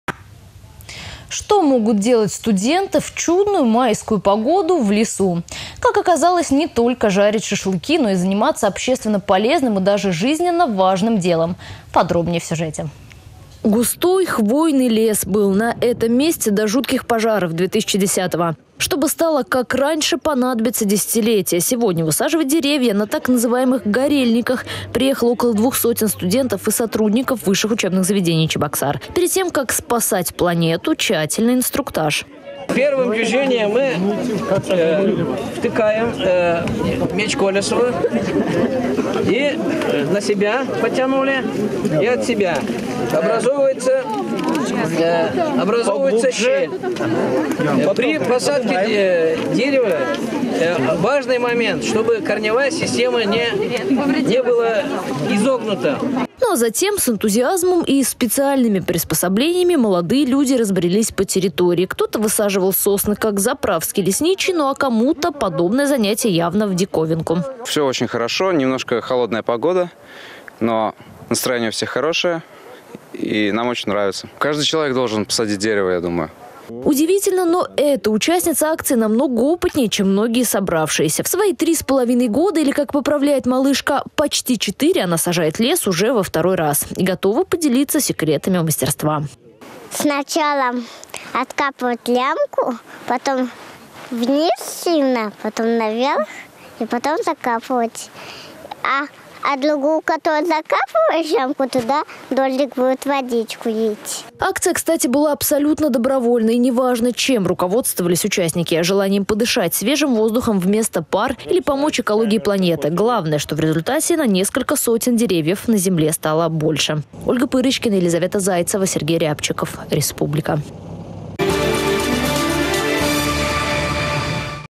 В Заволжье стало на несколько сотен деревьев больше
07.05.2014
Что могут делать студенты в чудную майскую погоду в лесу? Как оказалось, не только жарить шашлыки, но и заниматься общественно полезным и даже жизненно важным делом.